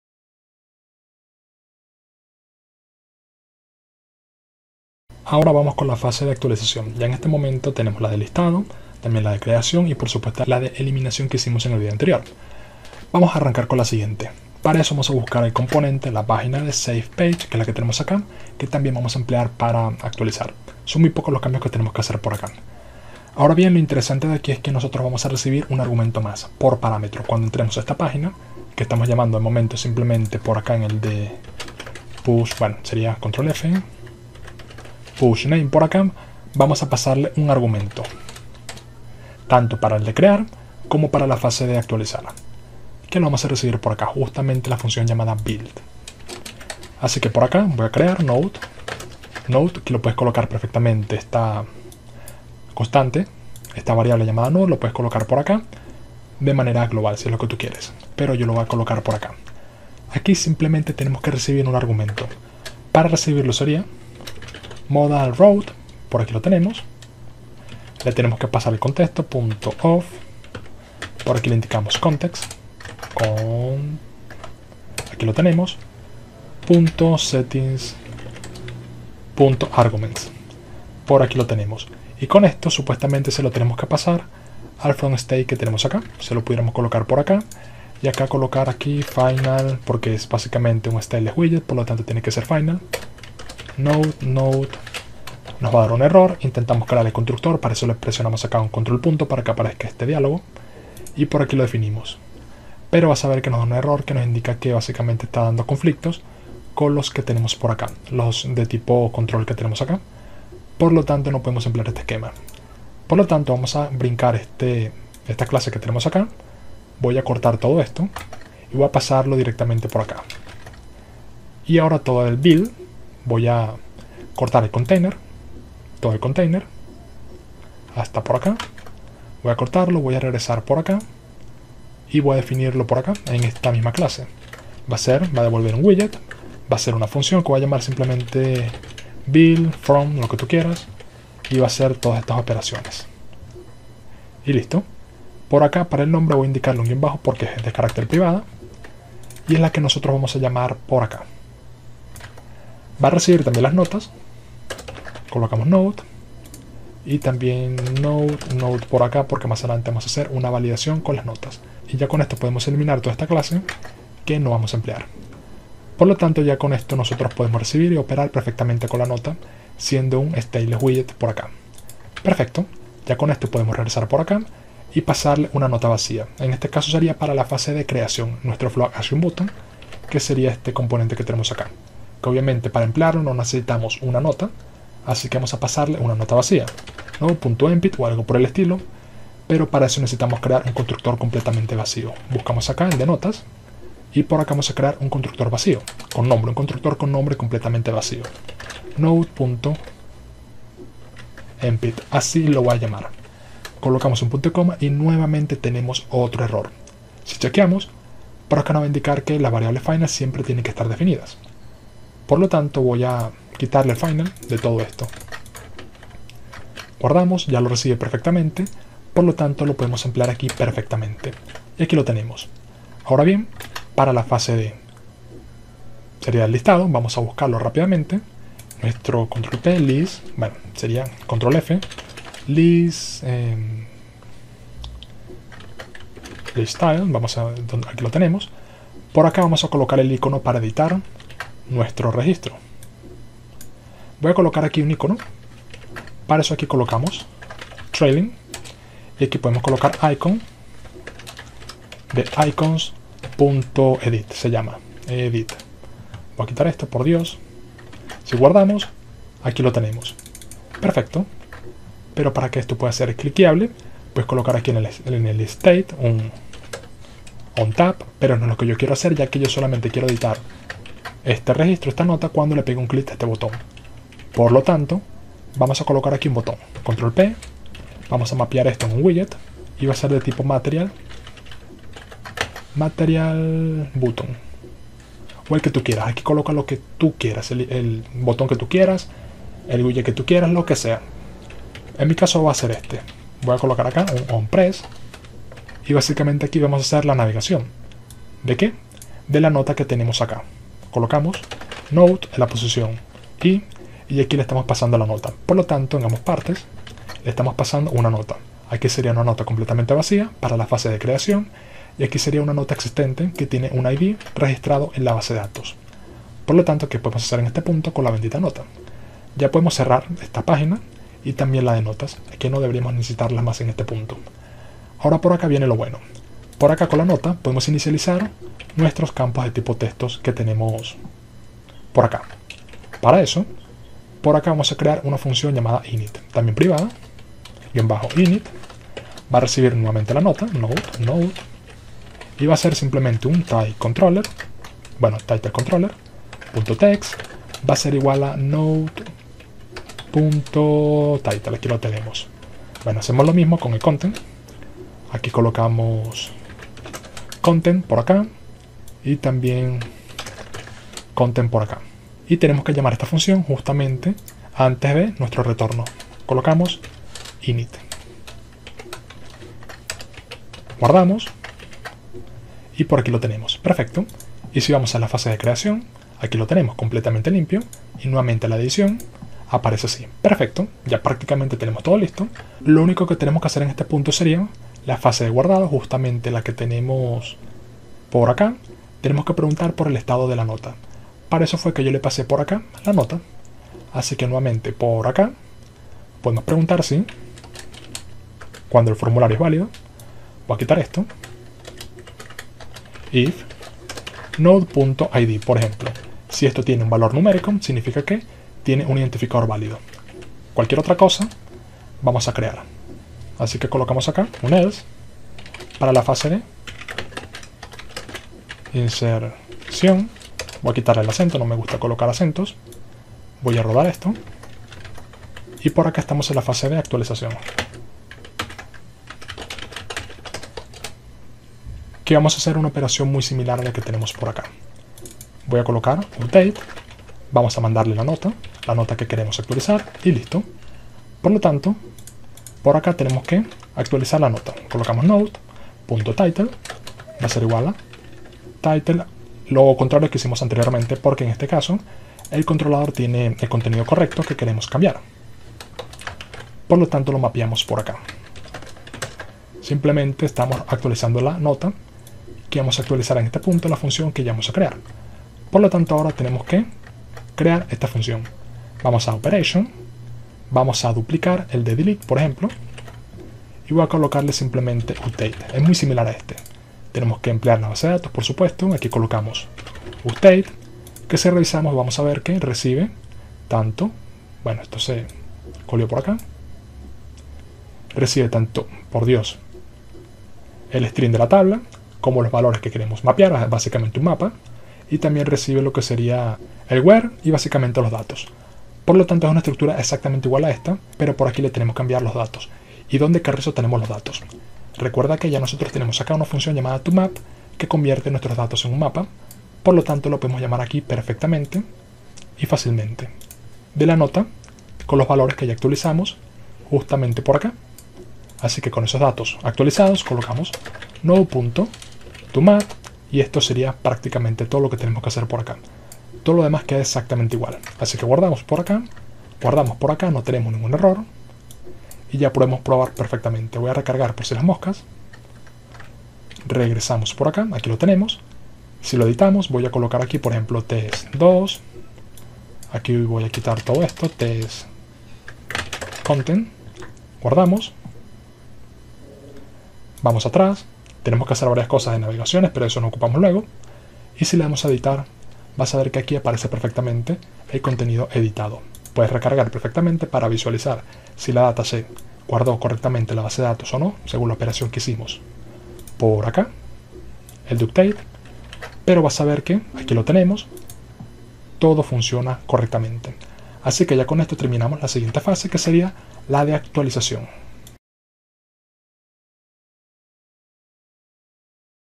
18 🚀 Actualizar (CRUD): Actualizar un registro en SQFlite con un formulario - Flutter
🚀 Vamos a aprender a crear un proceso de actualización mediante la misma página de creación.

Quieres una formación mas completa? prueba con mi curso COMPLETO!!!